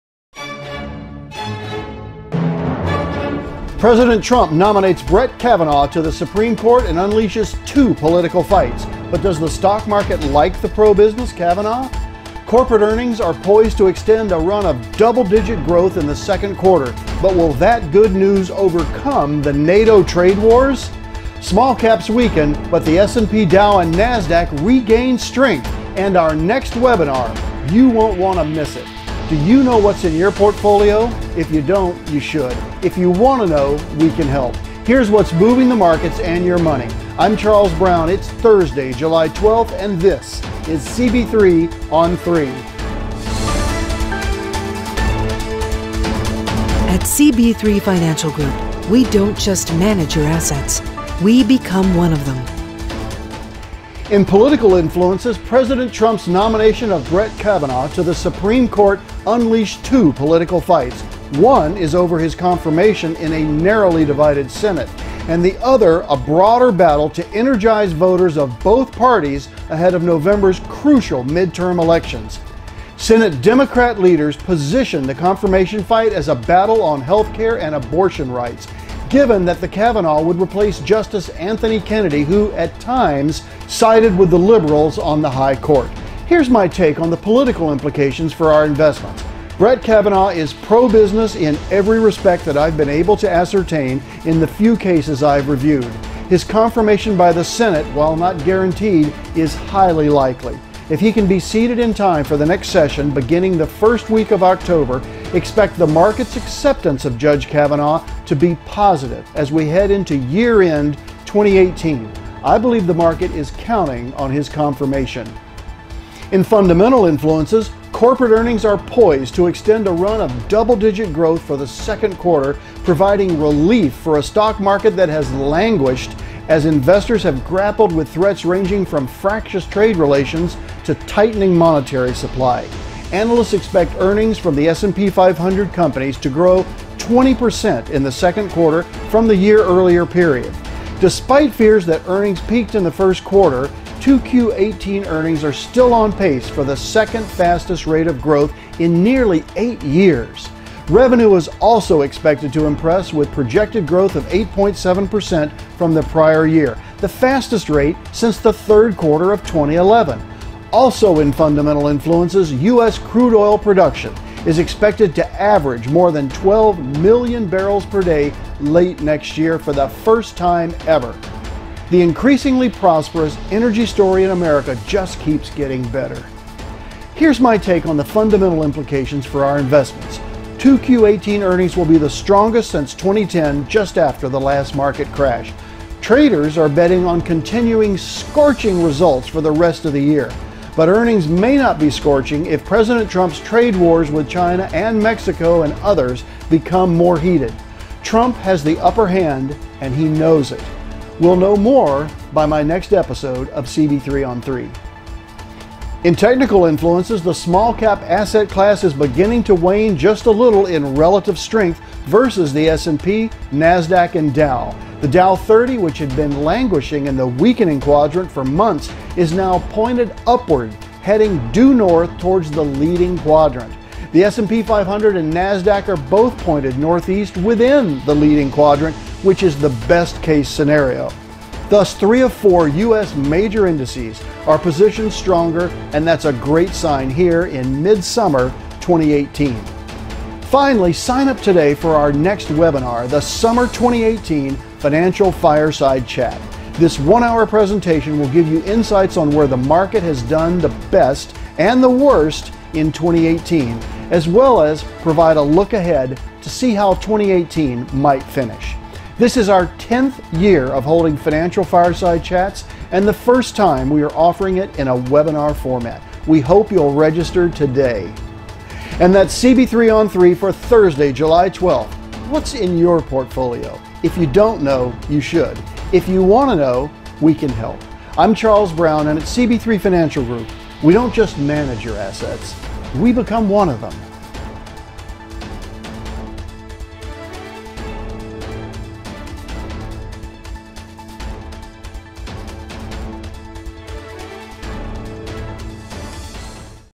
CB3 on 3 | Episode 035: Supreme Court Battle, 2Q18 Earnings
CB3 on 3 | Episode 035:

Market Vexation: Supreme Court Battle, 2Q18 Earnings, Energy Growth
Small Caps weaken, but DOW & S&P stronger